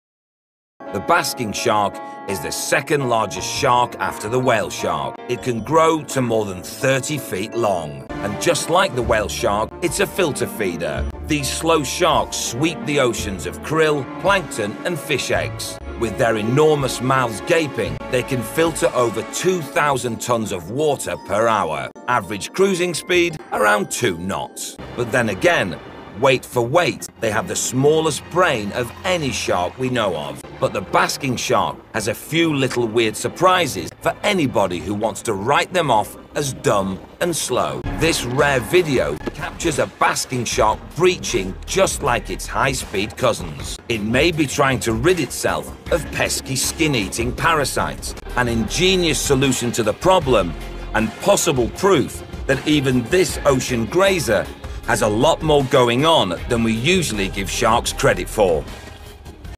why Basking Sharks are not really basking! 🤔
Contrary to its name, the basking shark isn't truly basking in the sun. Rather, it's a gentle giant of the sea, often found leisurely swimming near the water's surface, filtering vast amounts of plankton through its enormous mouth.